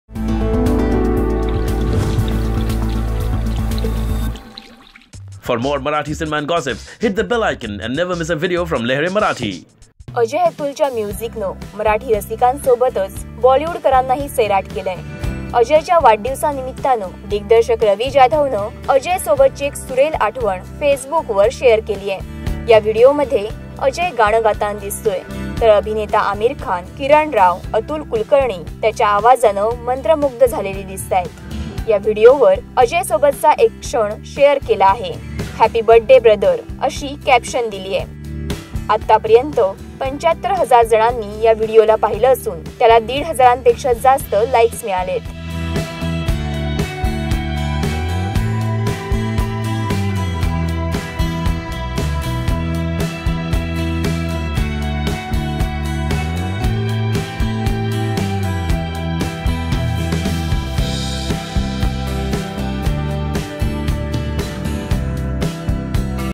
Ravi Jadhav Shares Special Moments With Ajay-Atul And Aamir Khan | Lehren Marathi
Ravi Jadhav shares a special moments with Ajay-Atul and Aamir Khan. If you want to see the moment check out this video.